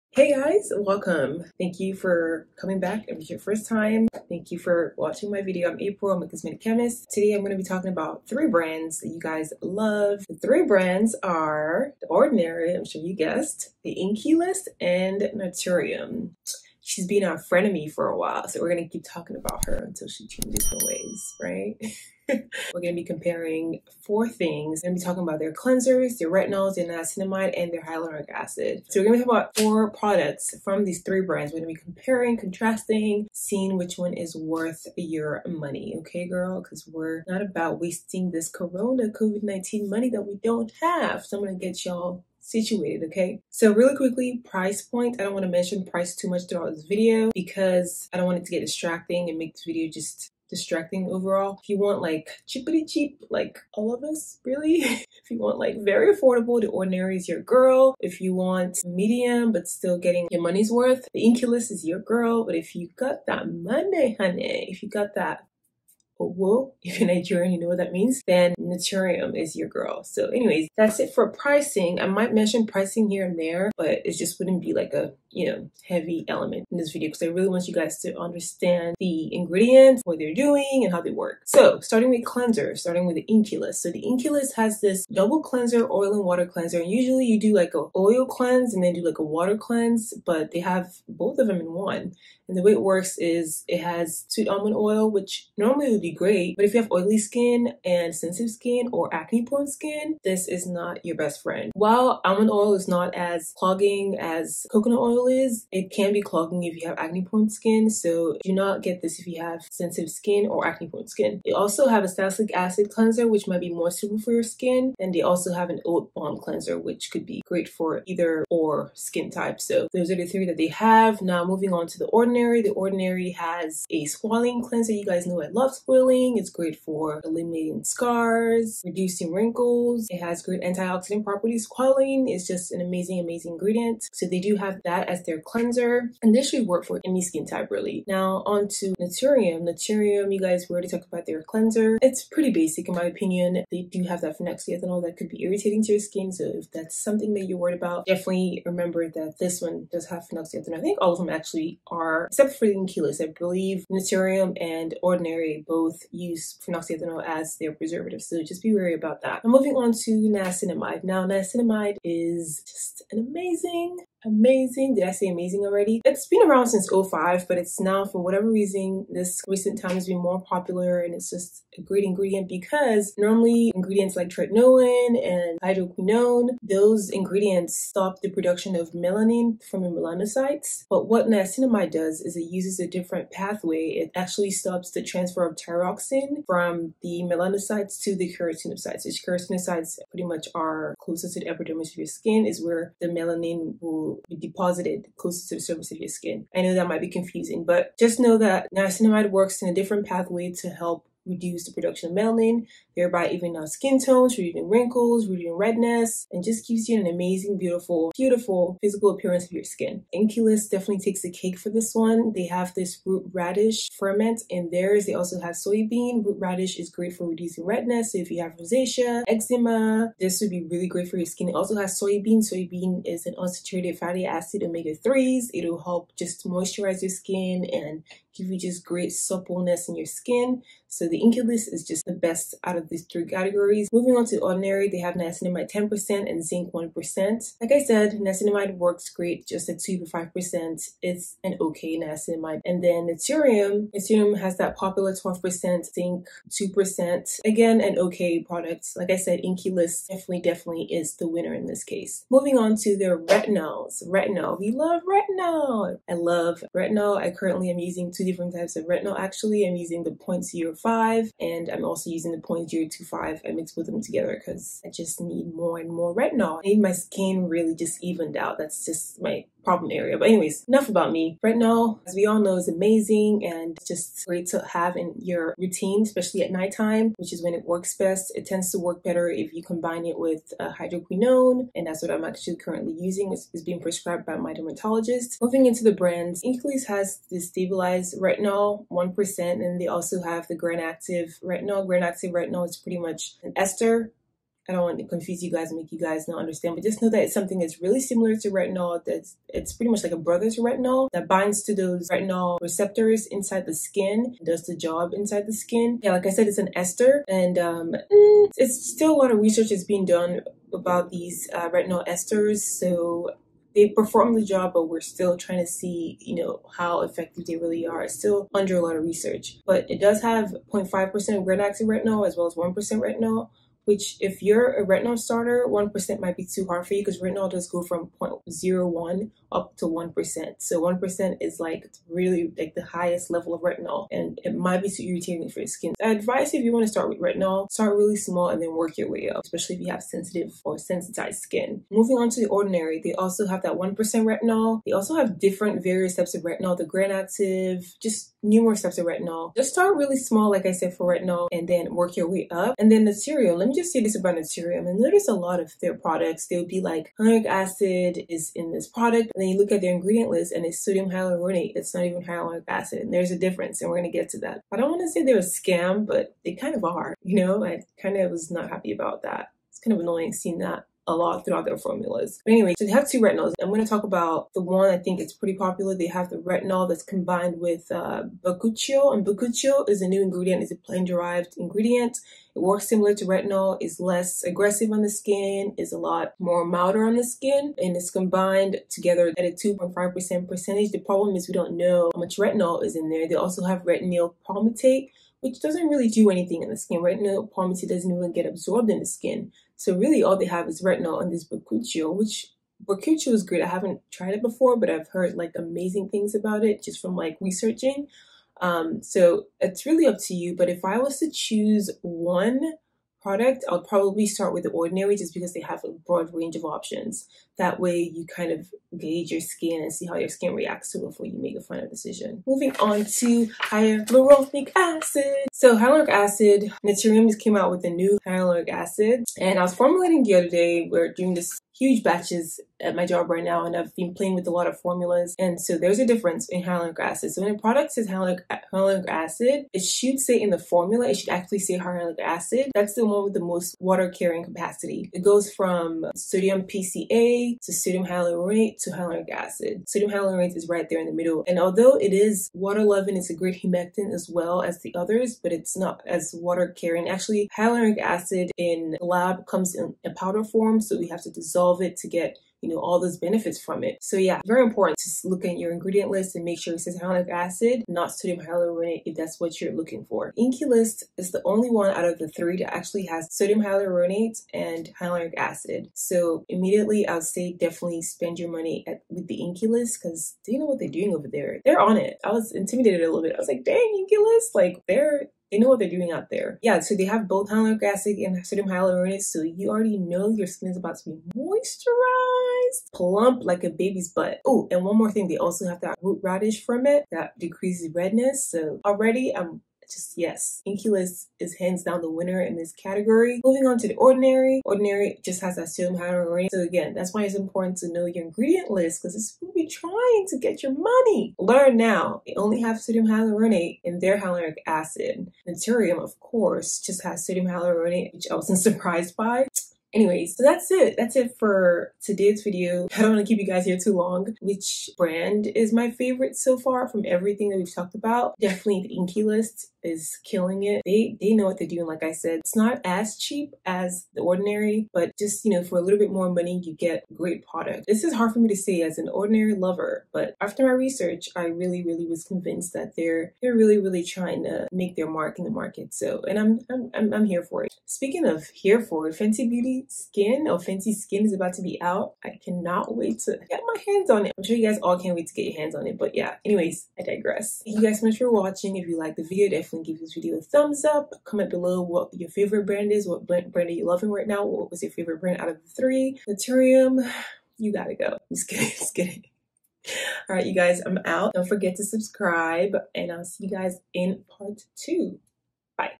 INKEY LIST vs THE ORDINARY vs NATURIUM Skincare Review | Battle of the brands | Chemists perspective
INKEY LIST vs THE ORDINARY vs NATURIUM Skincare Review... Battle of the brands | Chemist's perspective

In today's video, we have a battle of the brands, a juicy one. I will be reviewing 3 popular brands INKEY LIST vs THE ORDINARY vs NATURIUM. I will be comparing and contrasting their cleansers, retinoids, hyaluronic acid, and niacinamide. This will be part of the review. I will review 4 more products in part 2 of the video. I just didn't want to make a super long video.

 I will be going more into details about these products, their similarities, differences, and which one you should probably invest in.
I hope you like it! Please leave me a comment if you have any questions about anything or what else you would like to see, it really helps me know what direction to take my videos.

00:01:23 - Cleansers
00:02:40 - Niacinamide
00:04:13 - Retinol
00:07:03 - Hyaluronic Acid